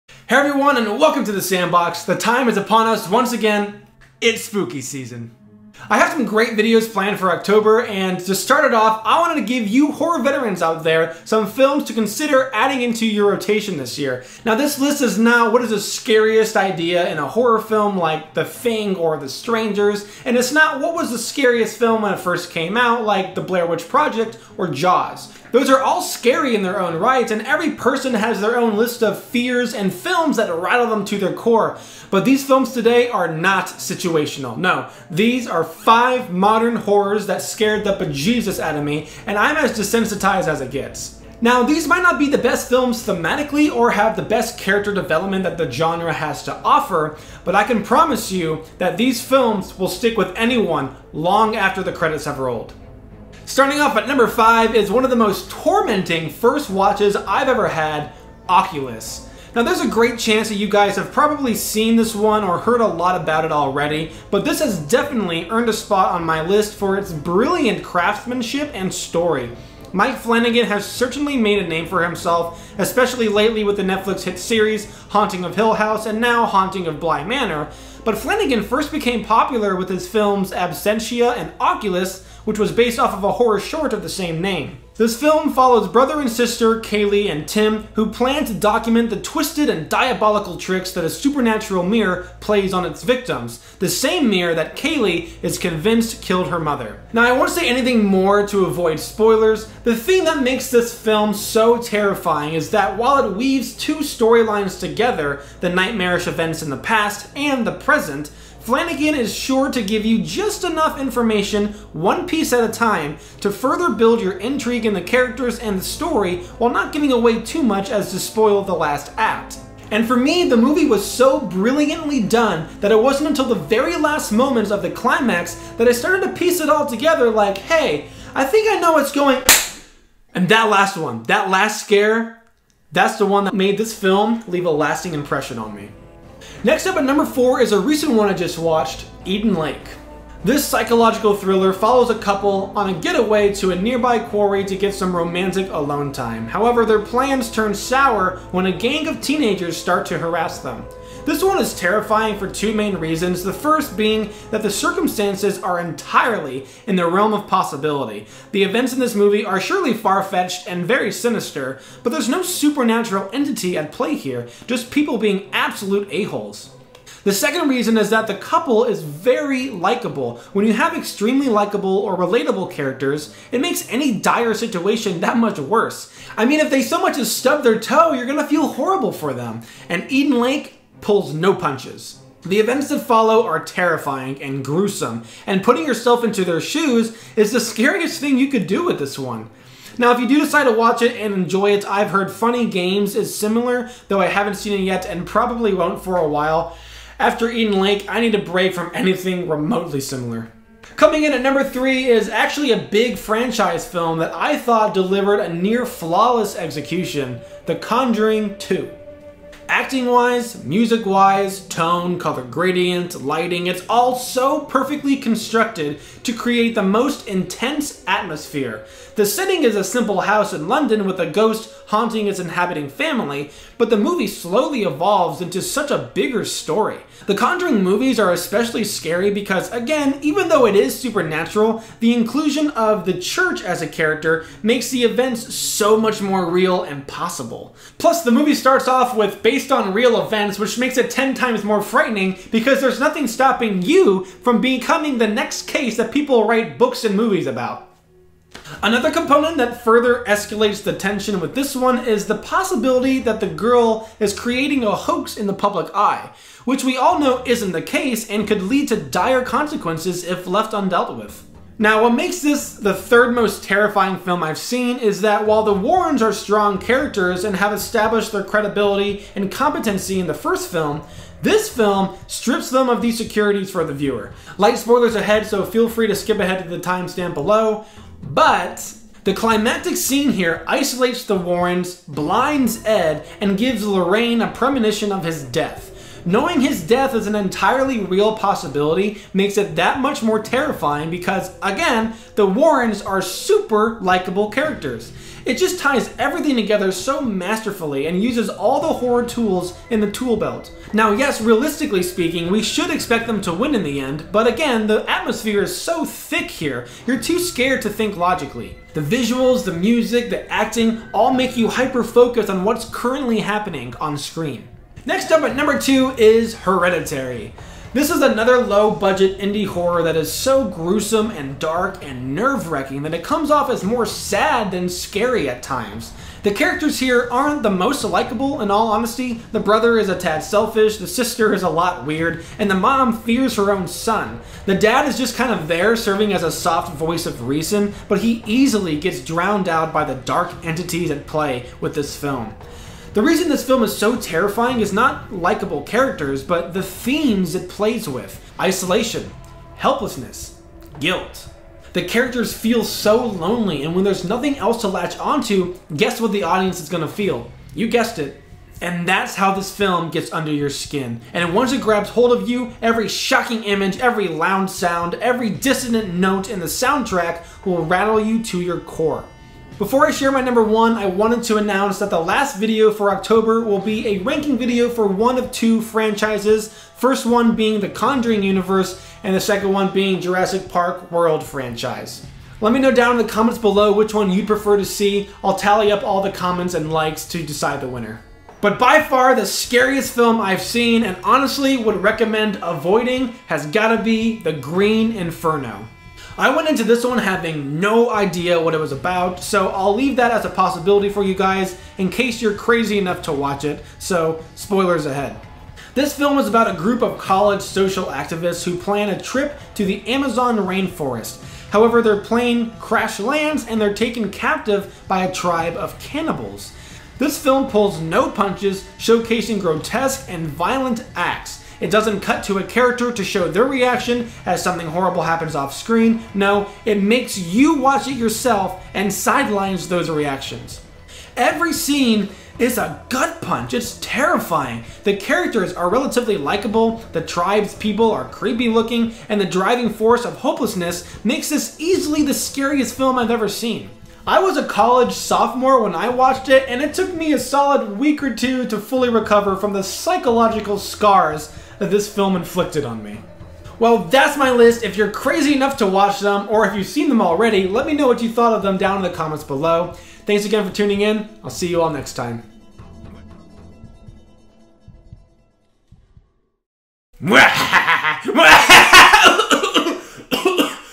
Top 5 Scariest Horror Films
🎃 IT'S SPOOKY SEASON!! 👻

This video is for all the horror veterans out there who struggle every year with finding the scariest movies to watch. Hopefully, there's one on this list you haven't seen, although I know there are a few that you most certainly have.